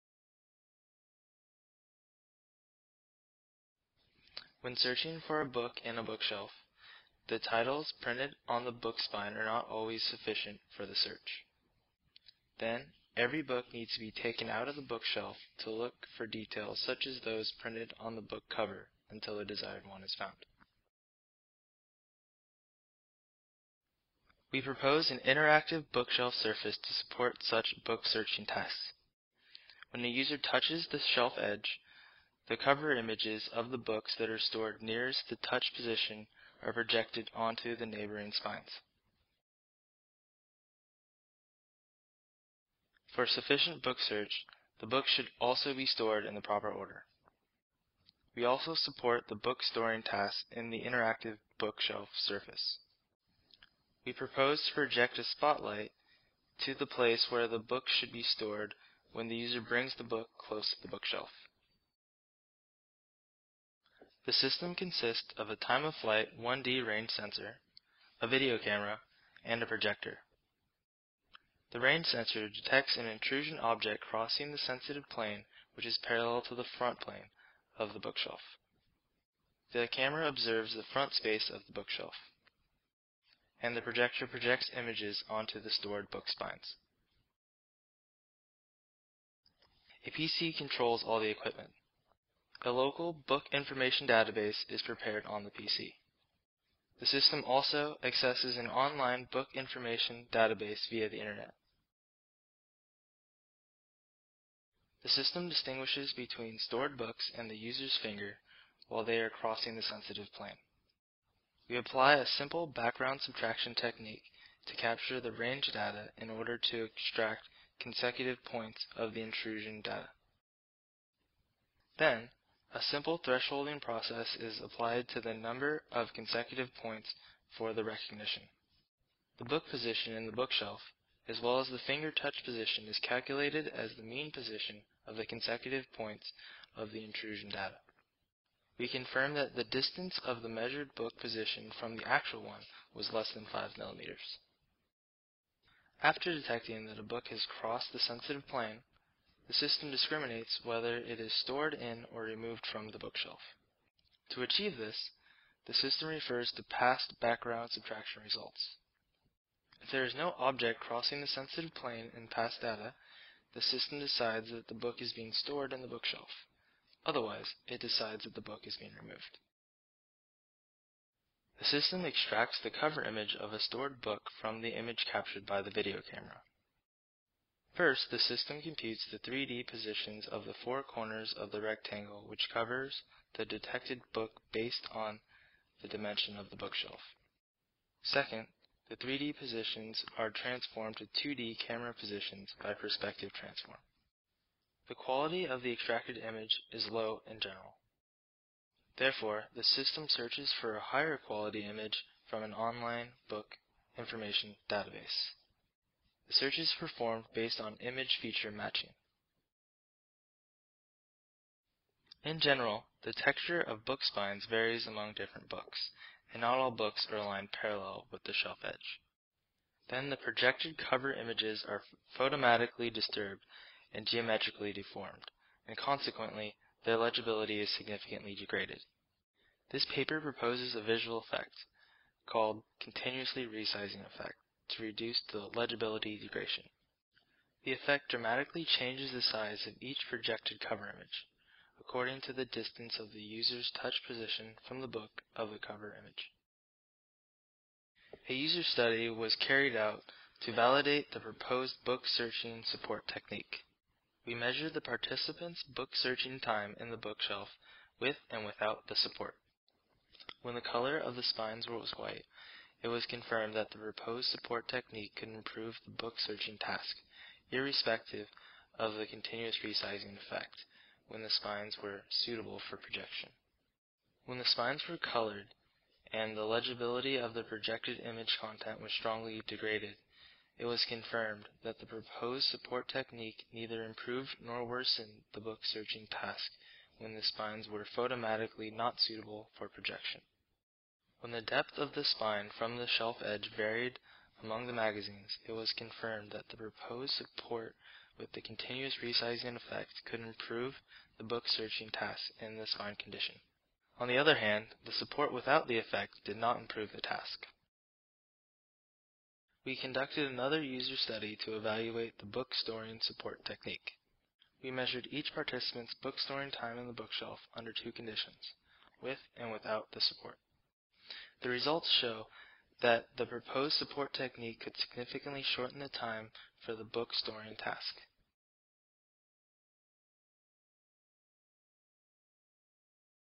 Interactive Bookshelf Surface
Kazuhiro Matsushita, Daisuke Iwai, and Kosuke Sato, "Interactive Bookshelf Surface for In Situ Book Searching and Storing Support," In Proceedings of Augmented Human International Conference, pp. 2:1-2:8, 2011.

Abstract:
We propose an interactive bookshelf surface to augment a human ability for in situ book searching and storing. In book searching support, when a user touches the edge of the bookshelf, the cover image of a stored book located above the touched position is projected directly onto the book spine. As a result, the user can search for a desired book by sliding his (or her) finger across the shelf edge. In book storing support, when a user brings a book close to the bookshelf, the place where the book should be stored is visually highlighted by a projection light. This paper also presents sensing technologies to achieve the above mentioned interactive techniques. In addition, by considering the properties of the human visual system, we propose a simple visual effect to reduce the legibility degradation of the projected image contents by the complex textures and geometric irregularities of the spines. We confirmed the feasibility of the system and the effectiveness of the proposed interaction techniques through user studies.